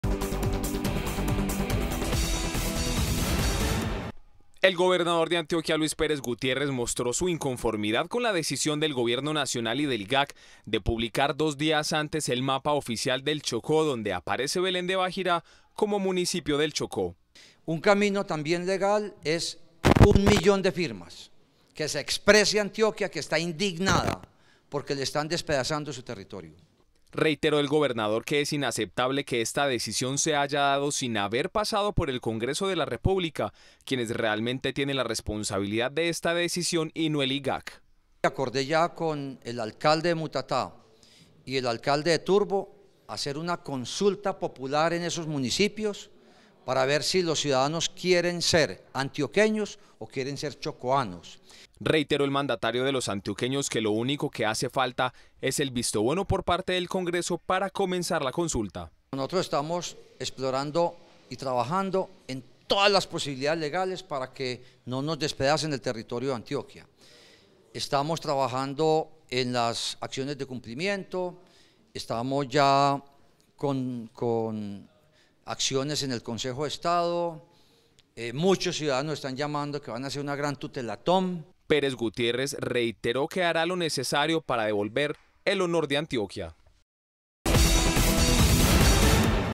Gobernador propone recolección de firmas para darle voz al pueblo
El Gobernador propuso además realizar una consulta popular en Belén de Bajirá.

-Teleantioquia Digital-